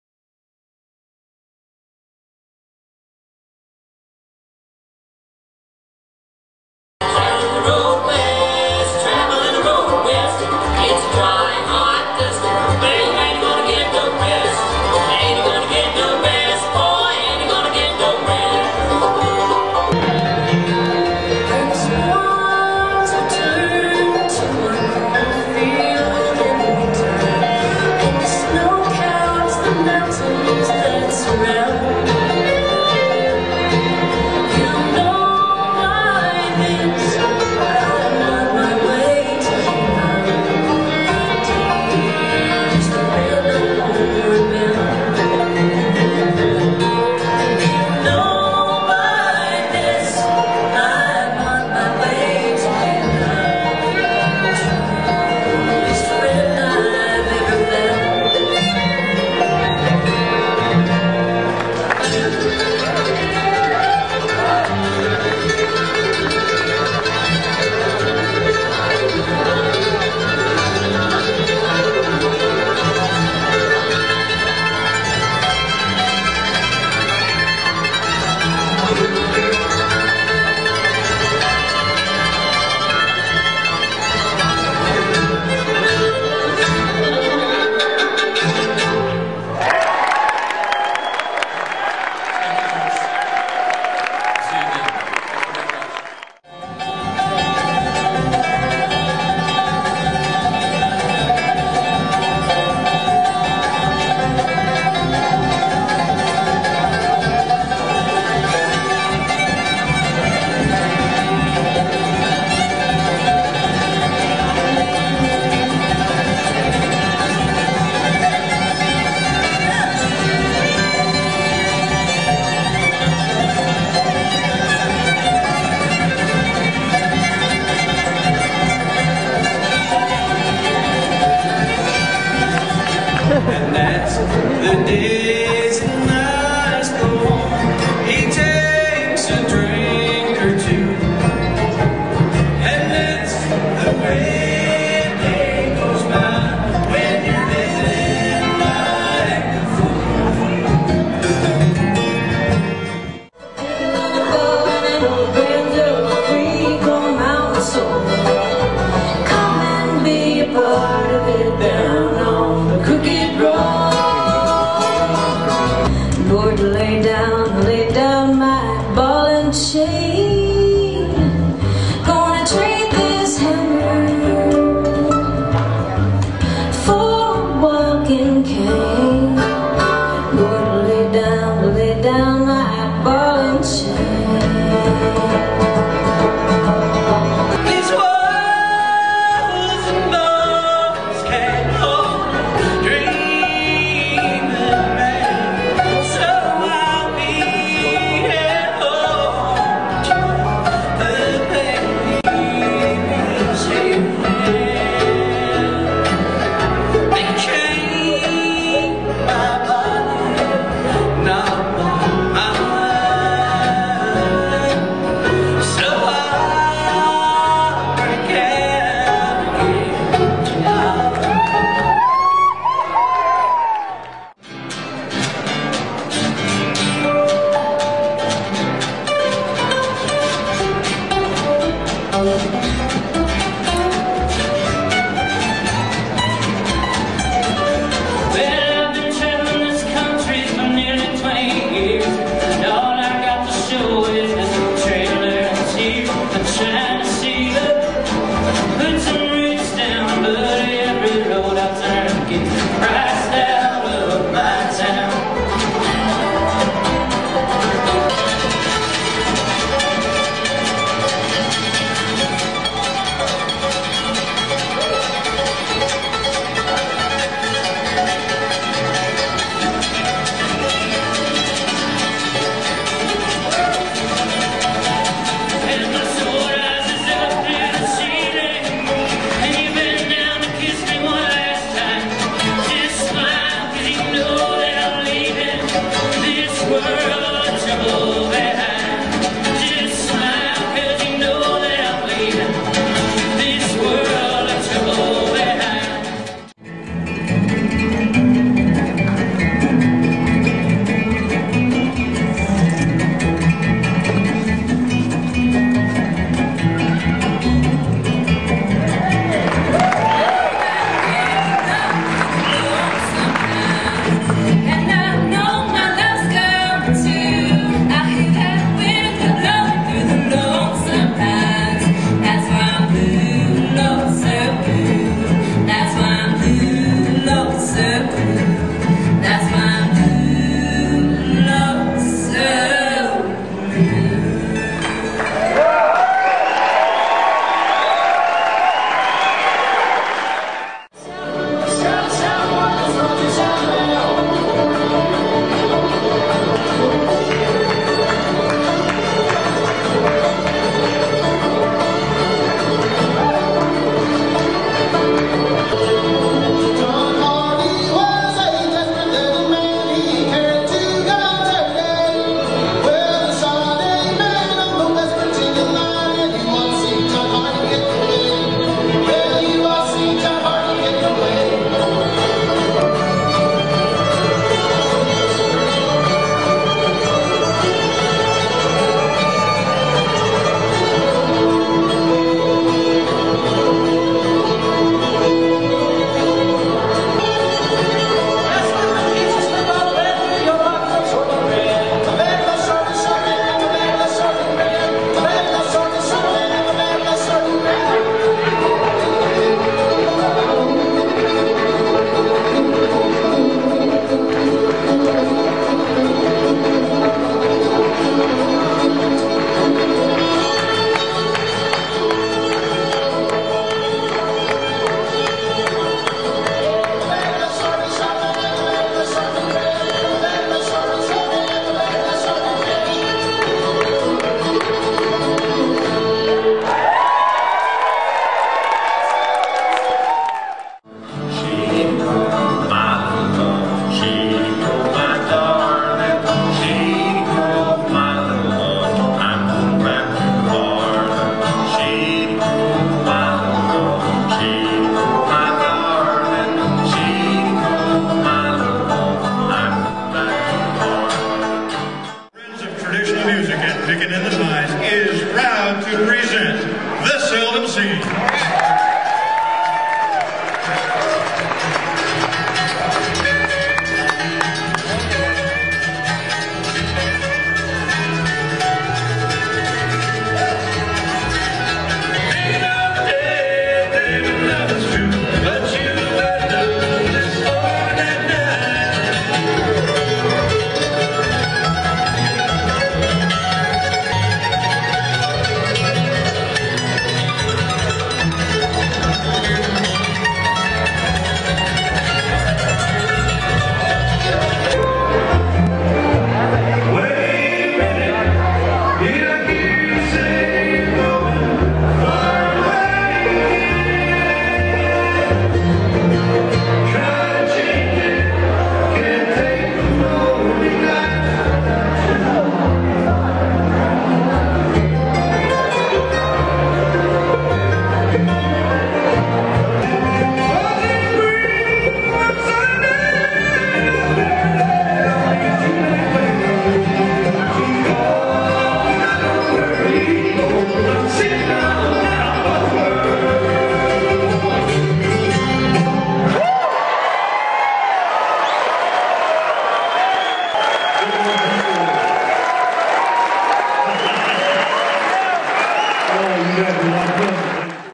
Pickin' In the Pines 2010 Sampler
Some highlights from Pickin' In the Pines Bluegrass & Acoustic Music Festival, held Sep 17-19, 2010, at the Pine Mt. Amphitheater in Flagstaff, AZ, organized by Flagstaff Friends of Traditional Music (FFOTM). You will see bits from the following:

John Reischman & The Jaybirds
Audie Blaylock & Redline
The Dixie Bee-Liners
Muskellunge Bluegrass
Whistle Stop
The Freight Hoppers
Tony Norris & Friends
The Seldom Scene

I'm sorry I was only able to attend Saturday and part of Friday.  I bet I missed a lot of other great performances!